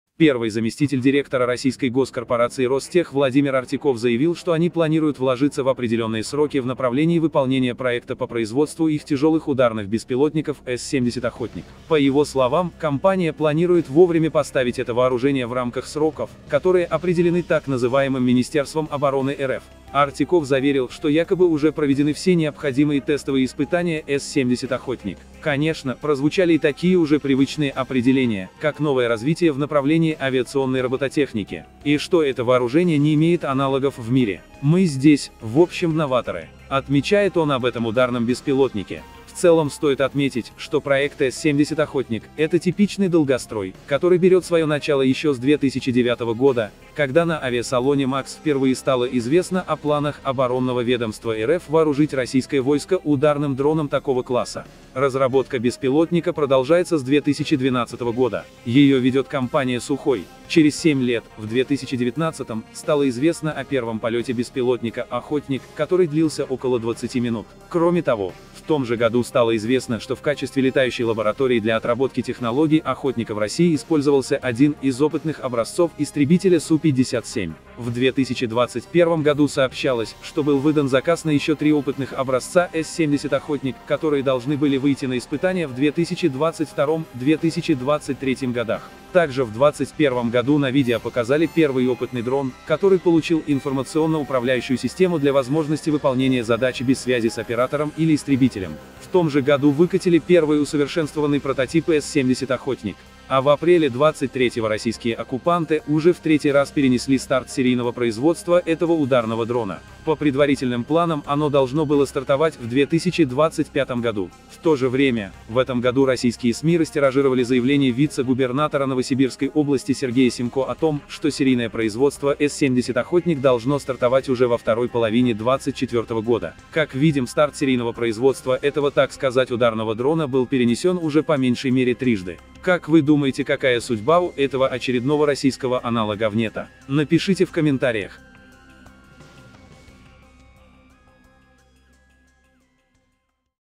В РФ уже 12 лет делают С-70 «Охотник» и заявляют, что все идёт согласно плану
Старт серийного производства этого ударного дрона был перенесен по меньшей мере трижды, а разработки по проекту «Охотник» продолжаются еще с 2012 года